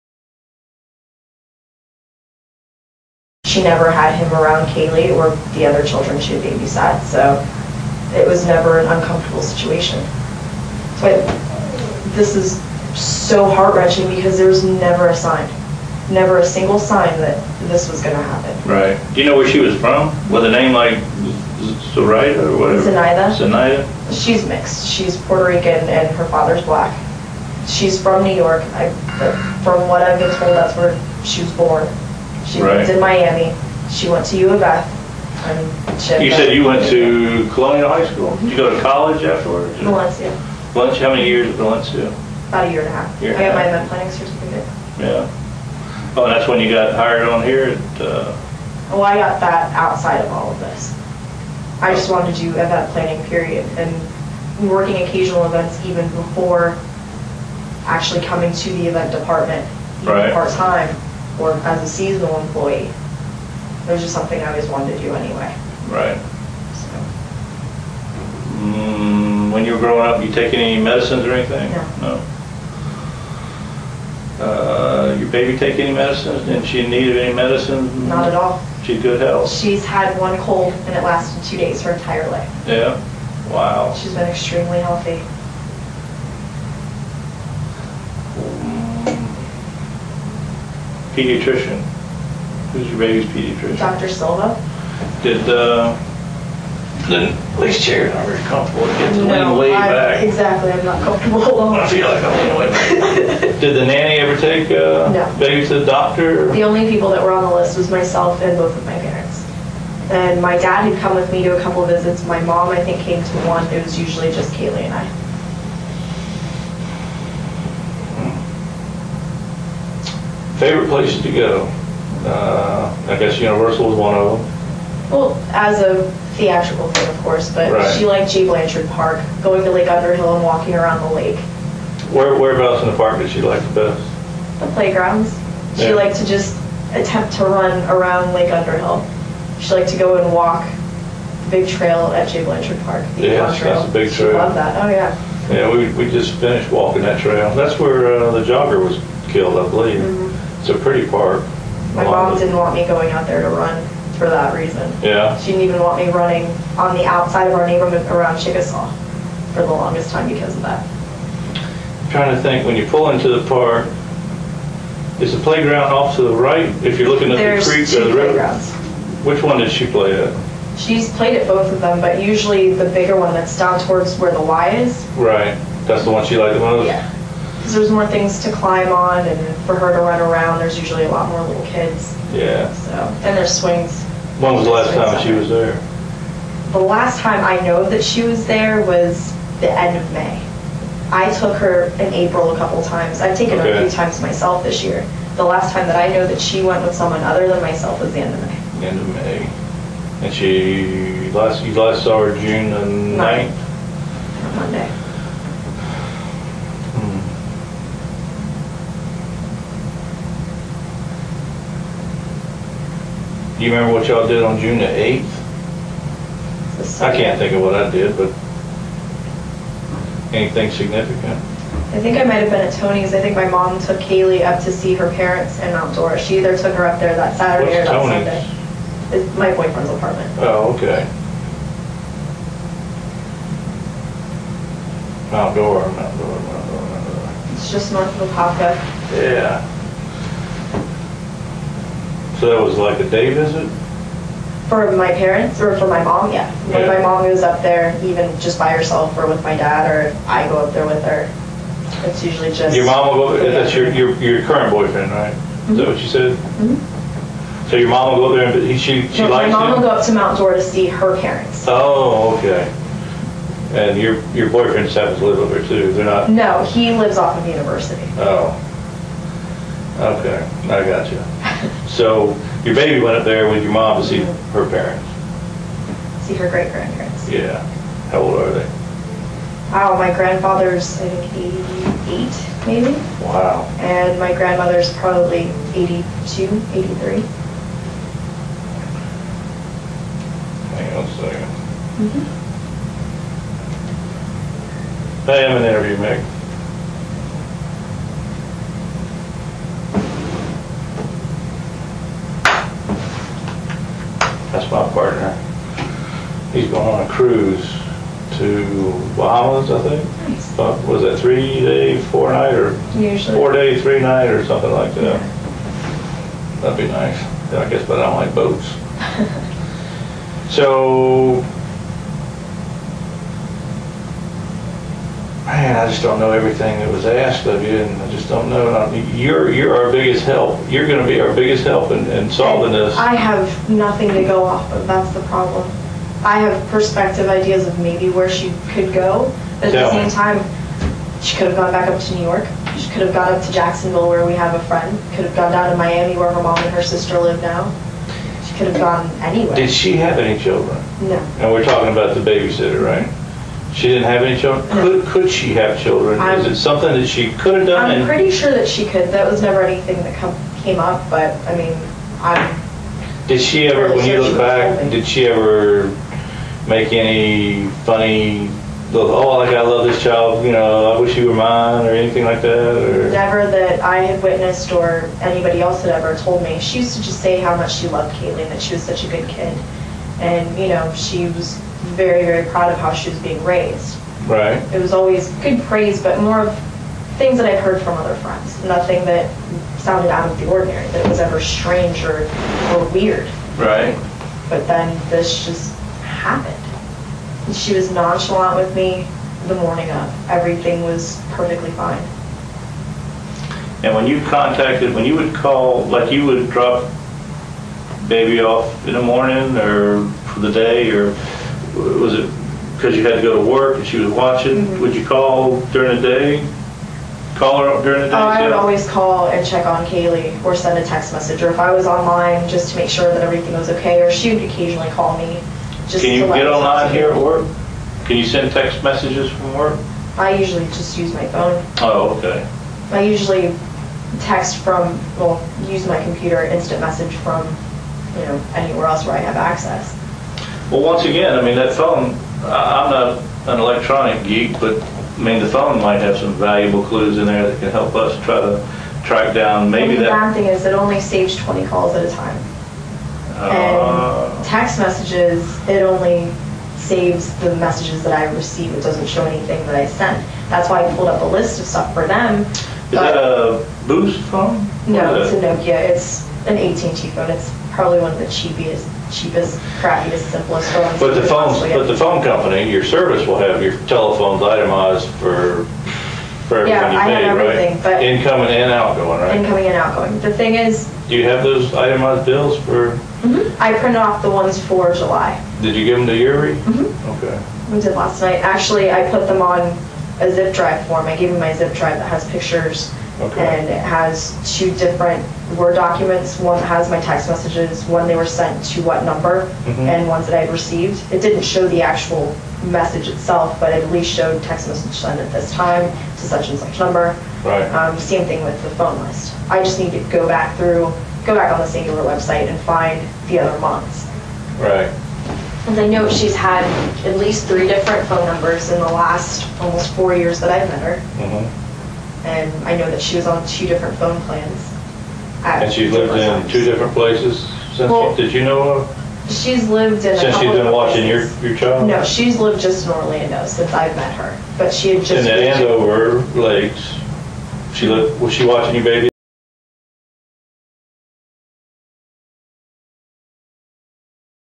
P7 - Casey Anthony's Police Interview Tapes Recorded at Universal Studios - Caylee Marie Anthony
Part 7 of 8.  Police interview Casey Anthony about the disappearance of her daughter, Caylee Marie Anthony.  These interviews occurred not long after the child was reported missing.  Casey told police she worked at Universal Studios and took them there to seek evidence.  Once they got there, after parading through corriders and so forth Casey finally admitted that she hadn't worked there for a while.  In fact, she had been fired in 2006 yet was still pretending to work there - to everyone, including her family.  Police took her into a conference room at Universal to conduct this interview.  It would be the last one they would get as Casey soon had a lawyer and would talk no more.